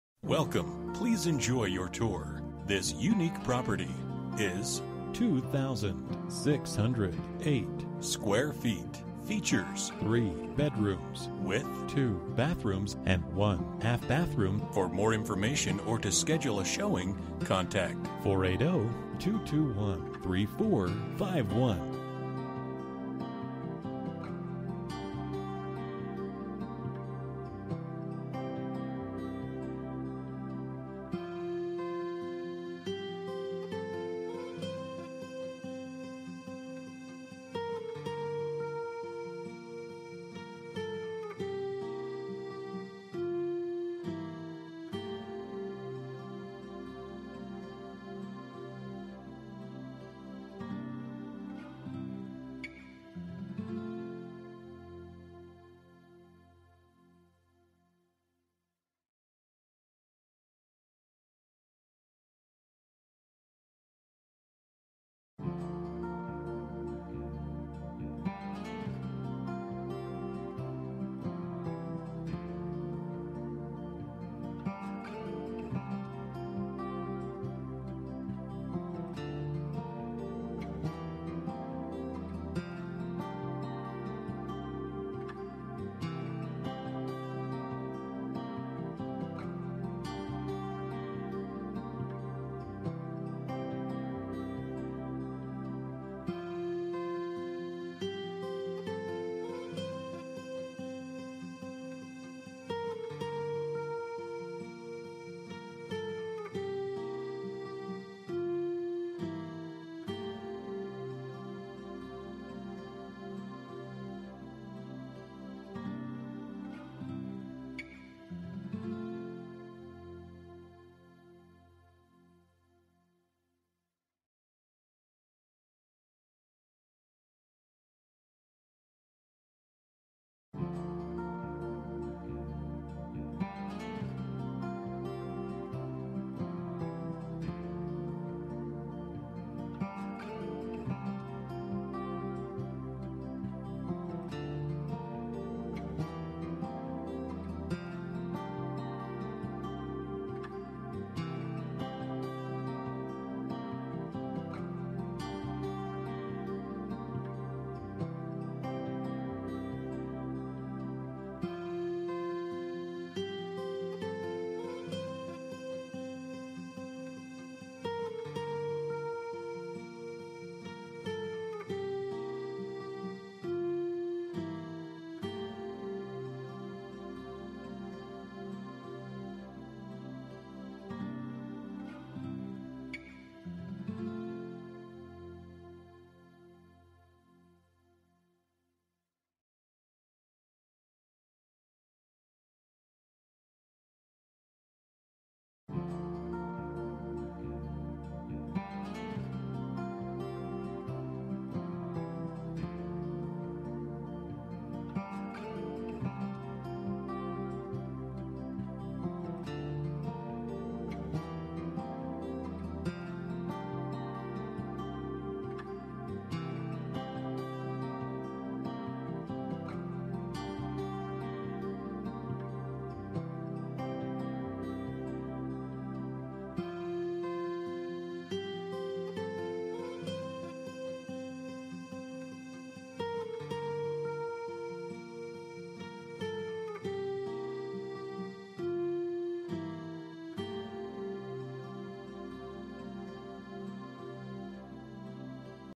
Residential for sale - 14208 N BURSAGE Drive, Fountain Hills, AZ 85268
WOW! What a wonderful opportunity to own this delightfully tranquil model perfect home in the desirable Sunridge Canyon subdivision in Fountain Hills! Boasting spacious and well-appointed rooms, this property features double door entry, Saltillo tile floors, family room with fireplace, inviting kitchen boasting large island, custom cabinets, 2 wall ovens, matching appliances, and breakfast area. The charming master bedroom showcases wood beam ceiling, private access to yard, and immaculate master bath featuring double sinks, separate tub and shower, and abundant wardrobe space. Would you believe, this home has a 2nd master bedroom/bath as well! The cozy yard is a lovely place to relax, has plenty of privacy, and a moving water pond. Check the UPGRADE list in Documents! A must see!

Bedrooms: 3
Bathrooms: 2 full and 1 half Bathrooms
Square Feet: 2608
Price: $449,000

For more information about this property, please contact Larry Rubino at or . You can also text 3346217 to 67299.